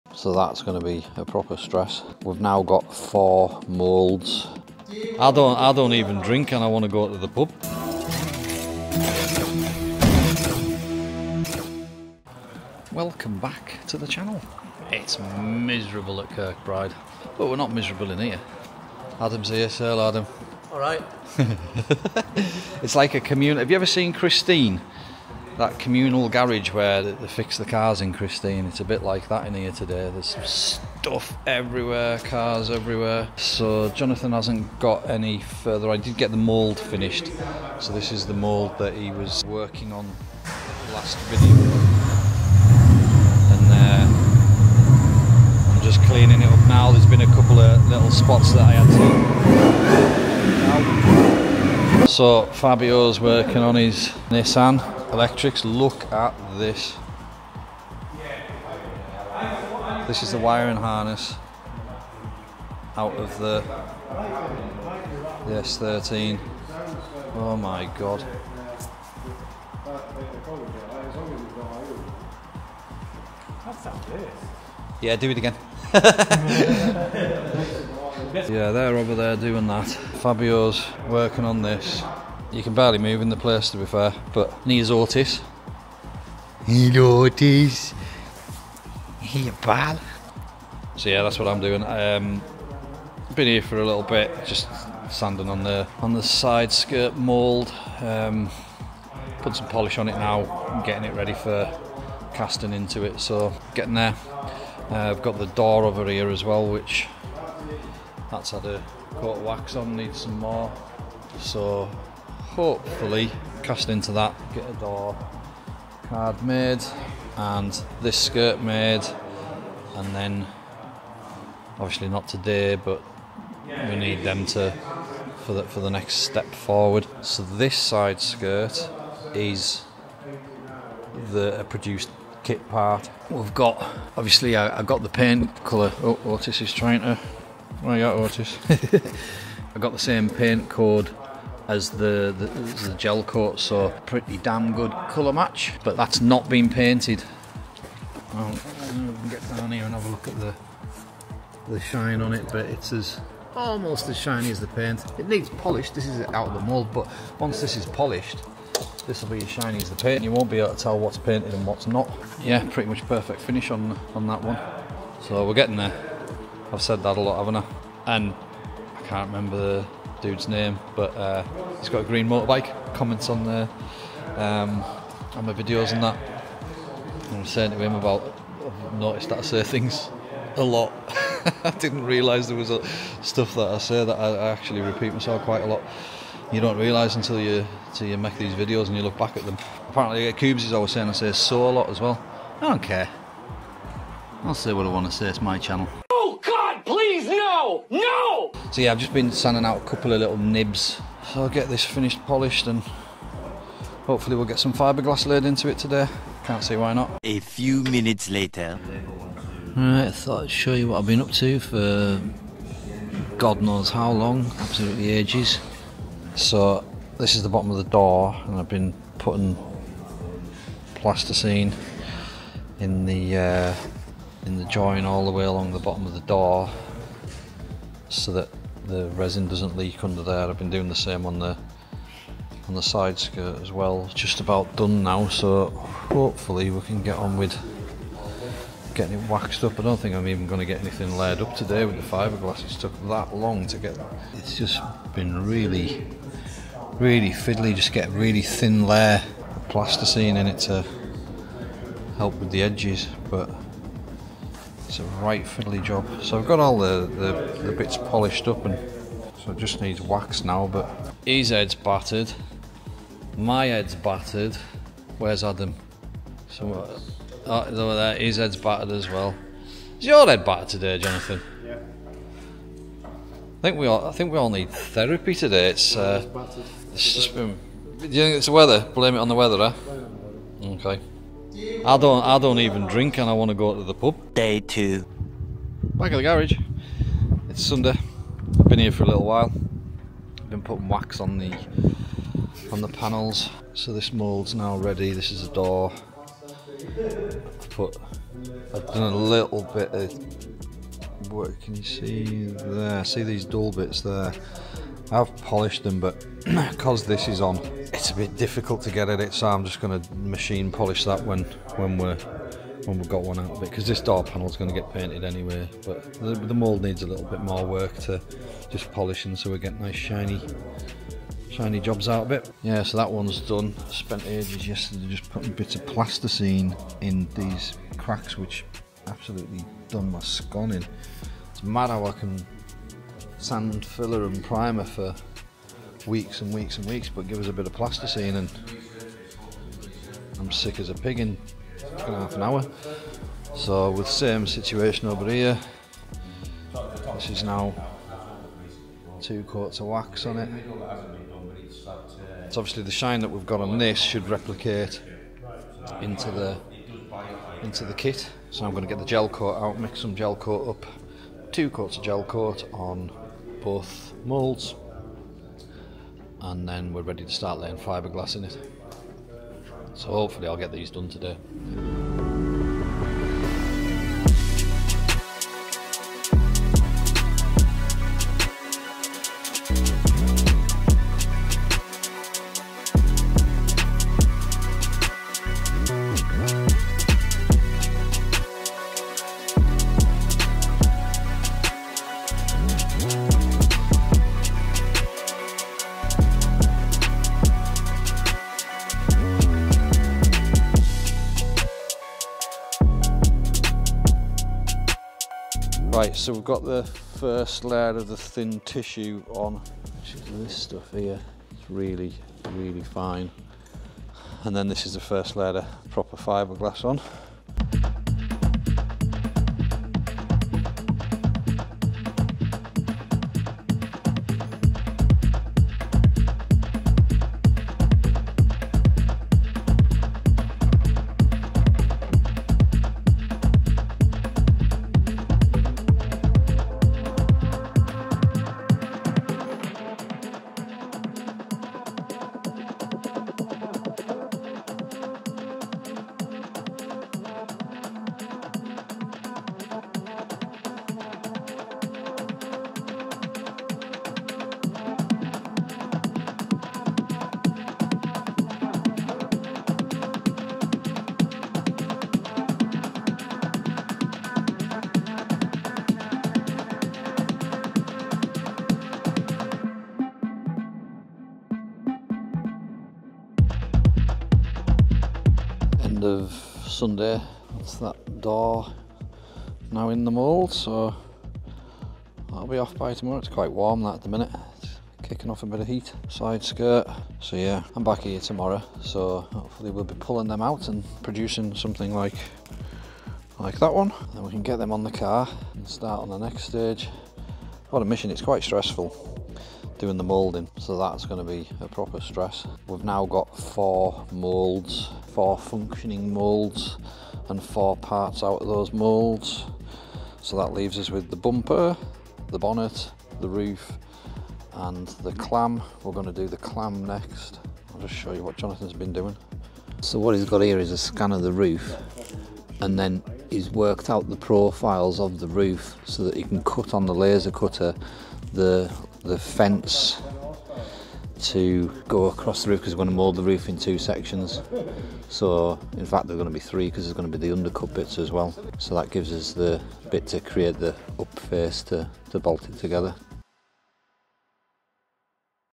Preparing moulds and making new fibreglass parts for the RB200 project
Cre8ors Podcast